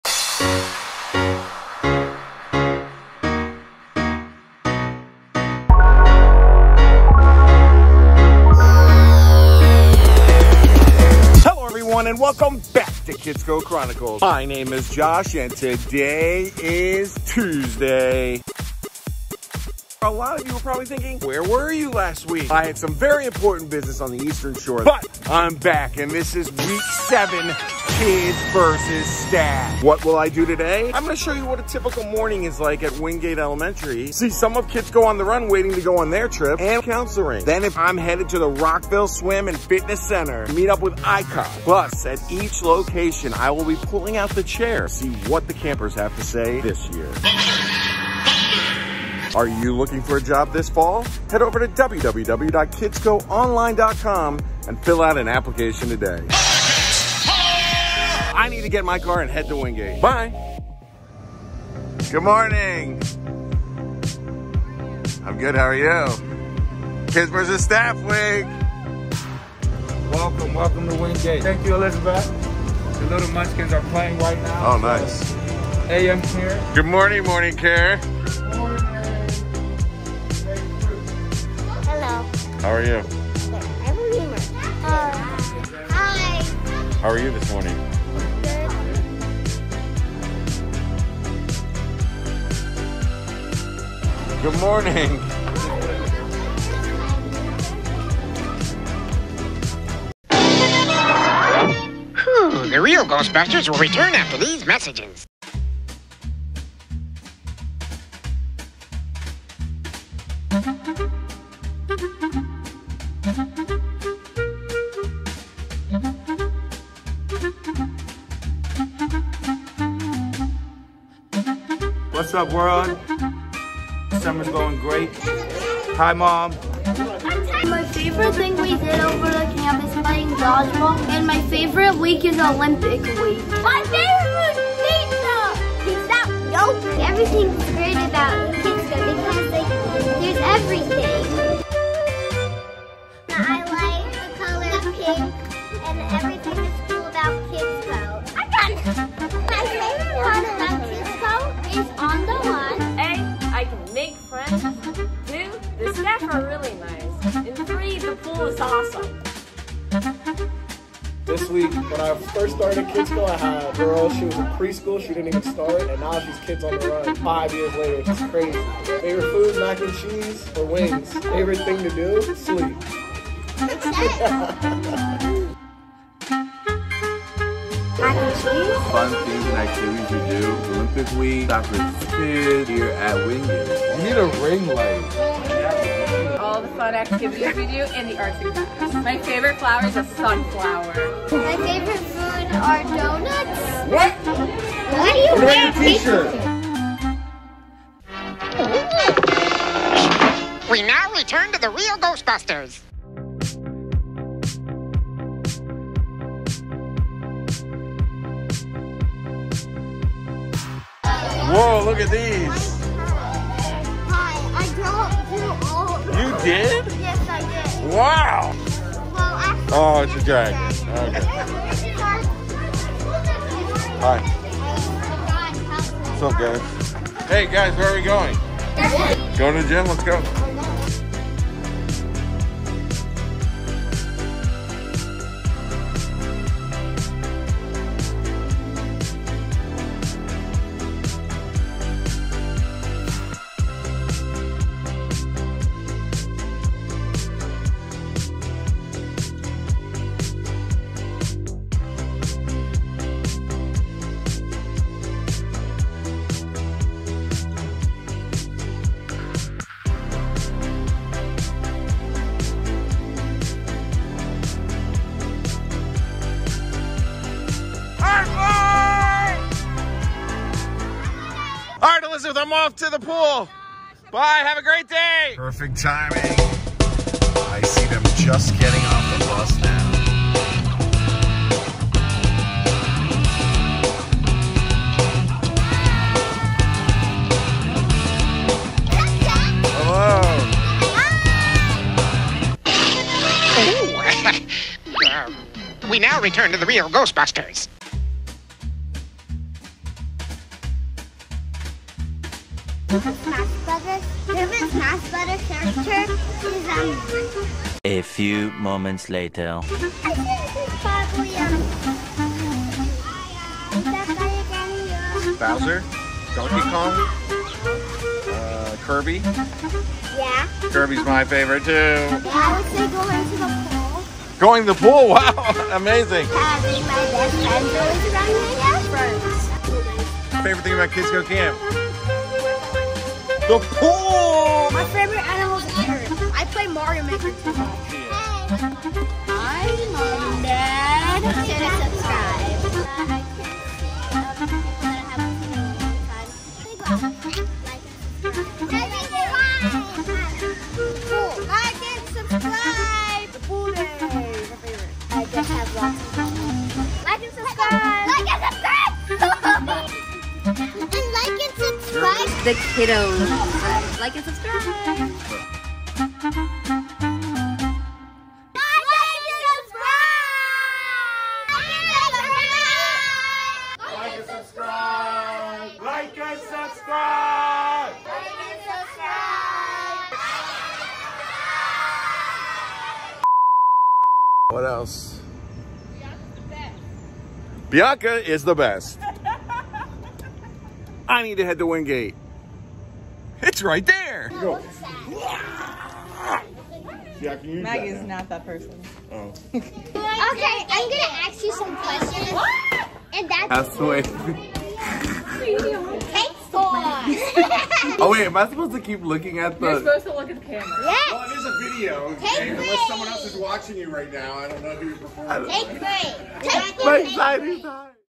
KidsCo Chronicles Episode 116
Welcome to week 7 of KidsCo Camps. This week's theme is Kids vs. Staff. Before we get to any events we start the morning off at Wyngate and then head to the pool with meet up with ICOG.